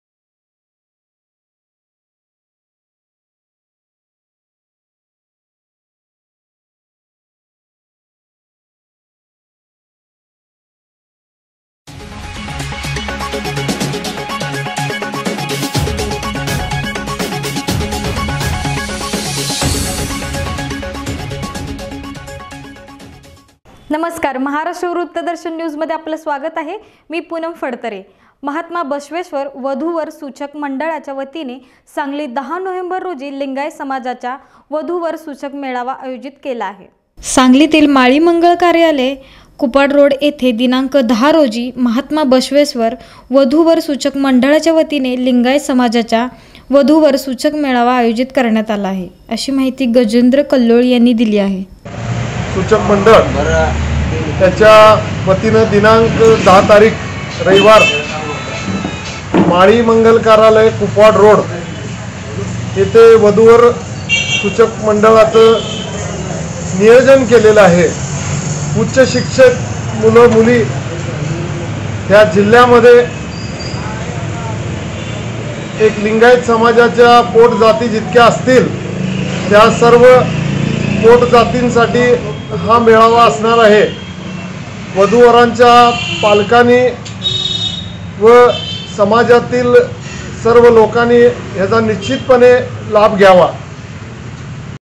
सांगली- १० नोव्हेंबरला लिंगायत समाज्याचा वधुवर सुचक मेळावा
माळी मंगल कार्यालय कुपवाड रोड येथे मेळावा.